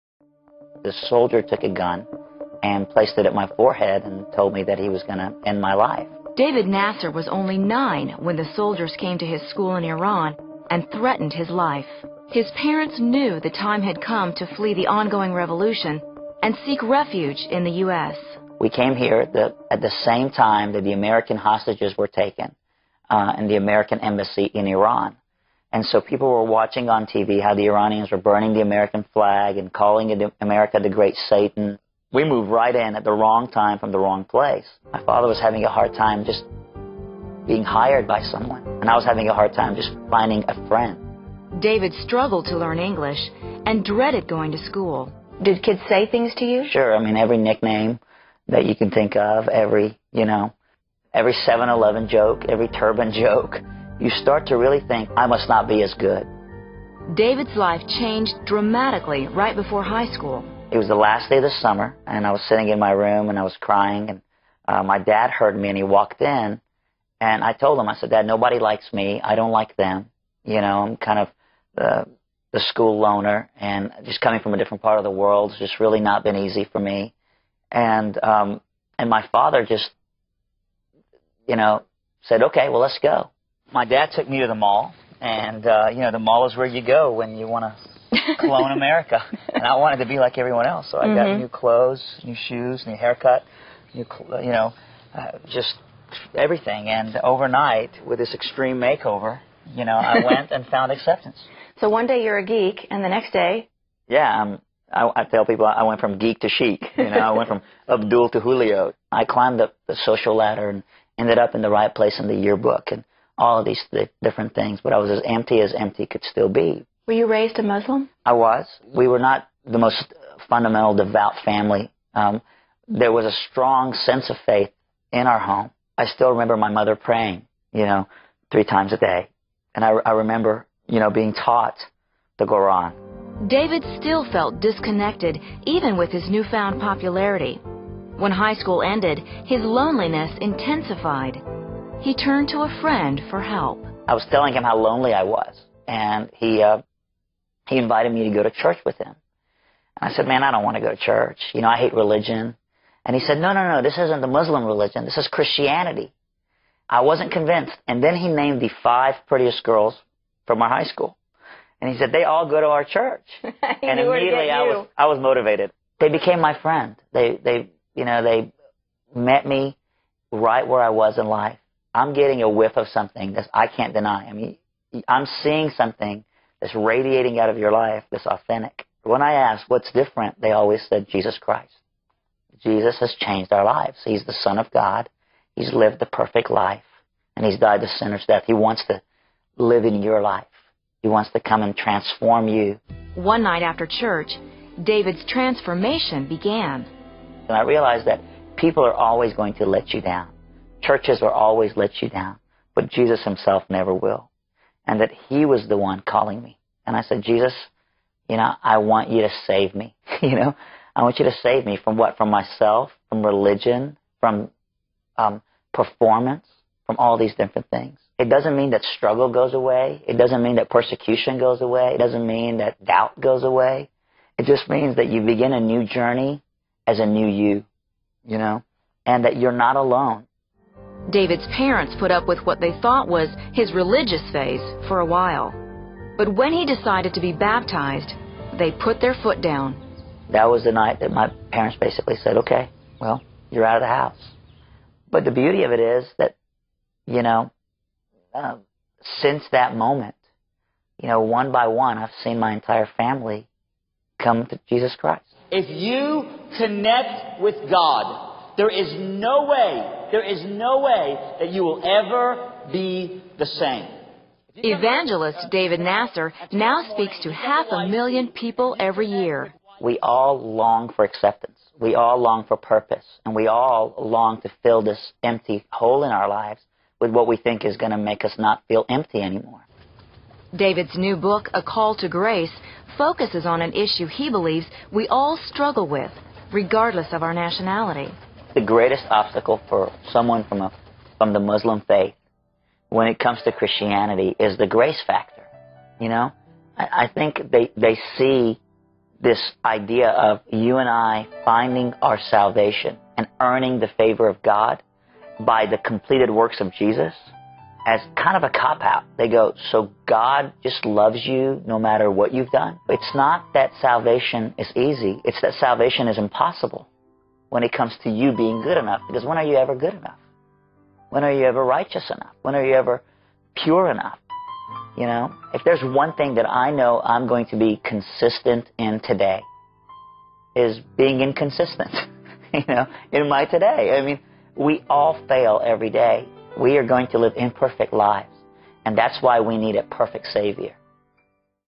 Former Muslim Boy Got Saved by the Grace - David Nasser
Today, he has a worldwide ministry but if you'd known him in high school, you wouldn't have looked twice at David Nasser. The former Muslim shares about his struggles in finding acceptance.